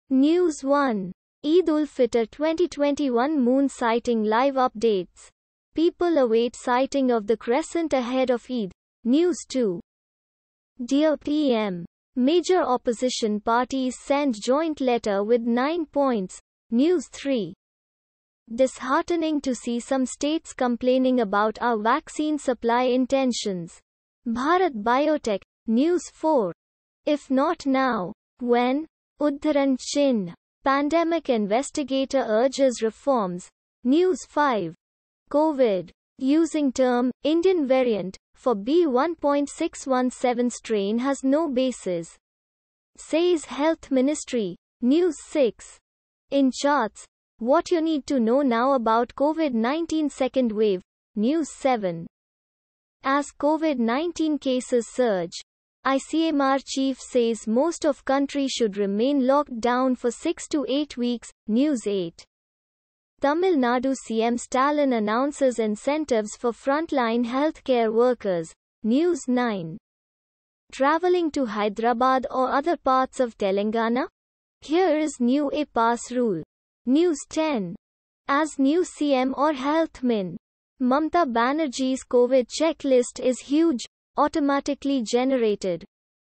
News Headlines (India)- 12/05/2021,08:00 PM
News 1:Eid-ul-Fitr 2021 Moon Sighting Live Updates: People await sighting of the crescent ahead of Eid, News 2:Dear PM: Major Opposition Parties Send Joint Letter With 9 Points, News 3:Disheartening to see some states complaining about our vaccine supply intentions: Bharat Biotech, News 4:"If Not Now, When?": Pandemic Investigator Urges Reforms, News 5:Covid: Using term 'Indian variant' for B.1.617 strain has no basis, says health ministry, News 6:In Charts | What You Need To Know Now About COVID-19 Second Wave, News 7:As COVID-19 cases surge, ICMR chief says most of country should remain locked down for 6-8 weeks, News 8:Tamil Nadu CM Stalin announces incentives for frontline healthcare workers, News 9:Travelling to Hyderabad or other parts of Telangana? Here is new e-pass rule, News 10:As New CM & Health Min, Mamata Banerjee’s COVID Check List Is Huge. Automatically Generated For Advertisement Contact